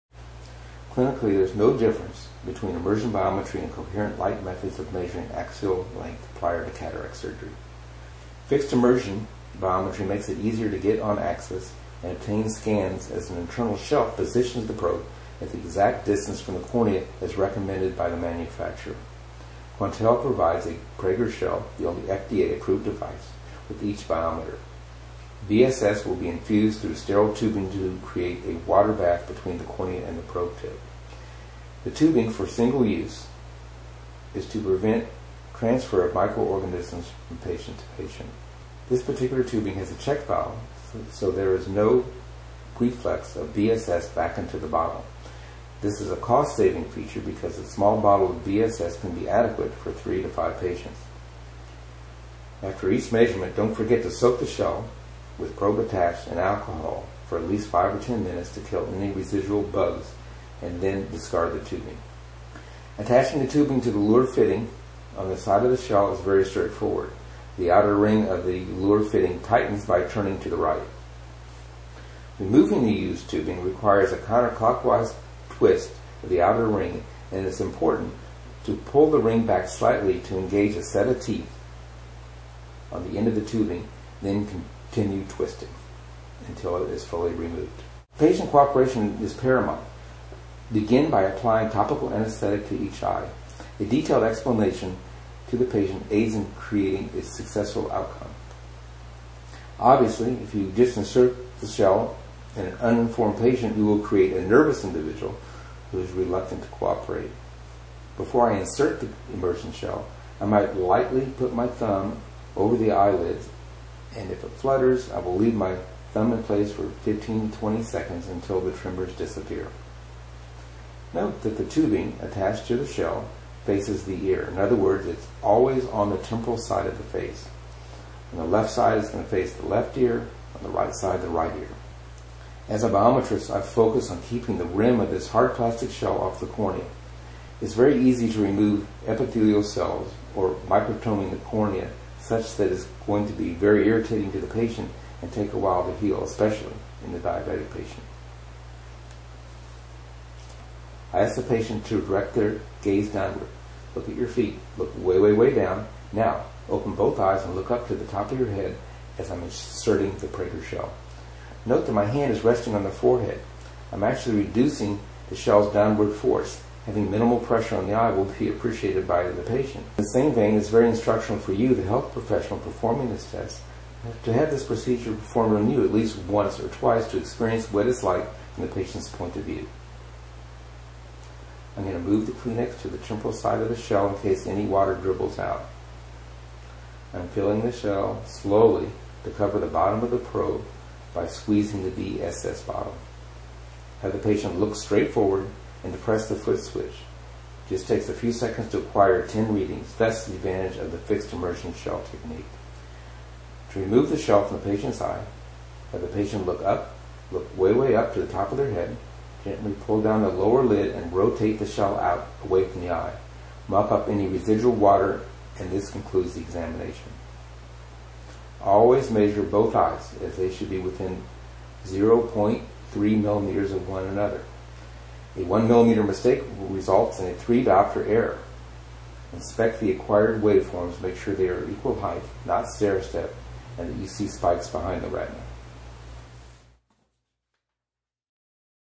ESI Part 07 - Demo of Prager Shell® for axial length measurements
EyeSurgin.com - The fixed immersion shell has essentially replaced the awkward two-handed open shell method of biometry. Clinically there is no difference between immersion biometry and coherent light methods of measuring axial length prior to cataract surgery. Fixed immersion makes it easier to get on axis and obtain scans as an internal shelf positions the probe at the exact distance from the cornea as recommended by the manufacturer.